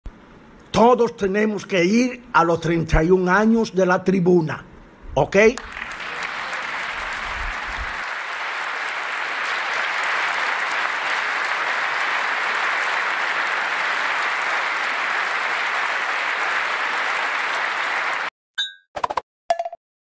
QUE LO SEPA TODO EL MUNDO CADA 27 de FEBRERO
La fotografía de LA UNIDAD 27 de Febrero del 2021, estadio Julián SFM. 2:30 P.M. La gran bendición e invitación con El PADRE, EL HIJO Y ESPÍRITU SANTO - 32 Años de La Tribuna Radio Tv. Ptor Brunildo Ant.